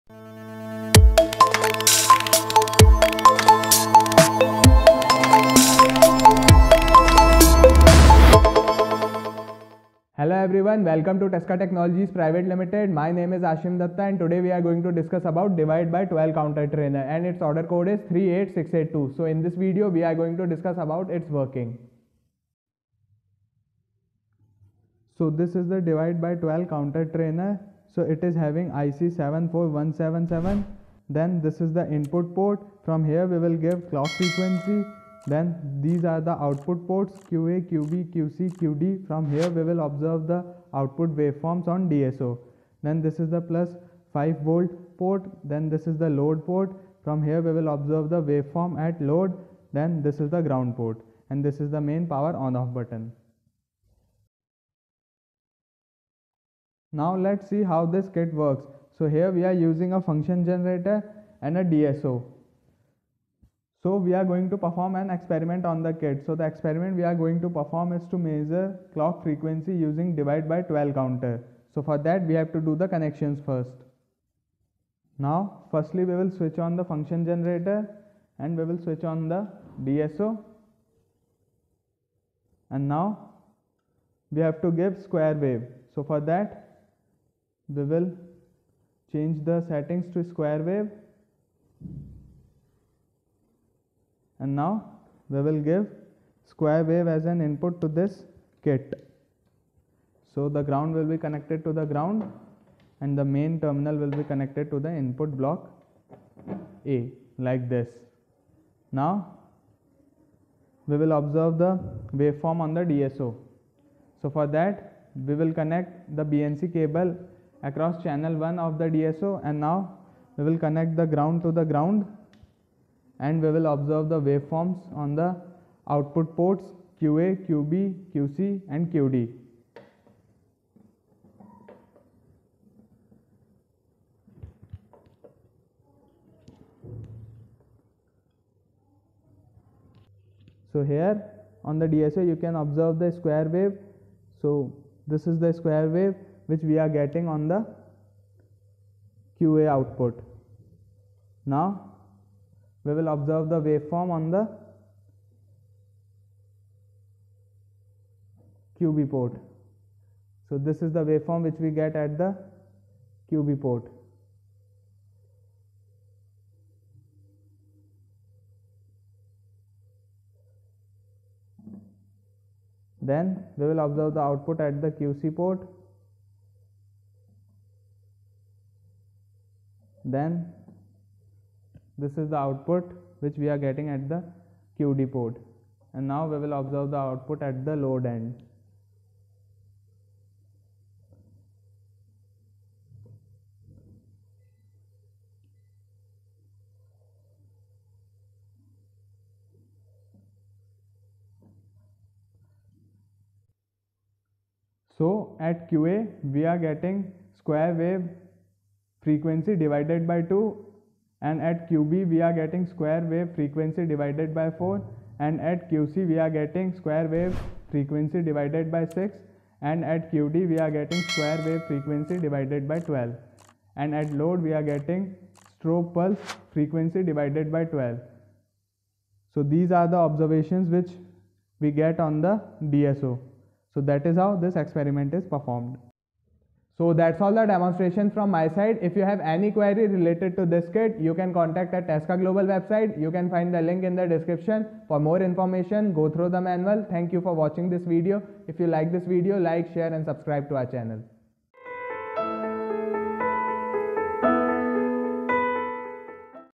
Divide by 12 - counter using IC-74177 | Digital Electronics Trainers | Tesca 38682
Divide by 12 - counter using IC-74177 is useful training/didactic equipment for learning concepts of Divide by 12-counter using IC-74177.

In this video, we described Divide by 12 - counter using IC-74177.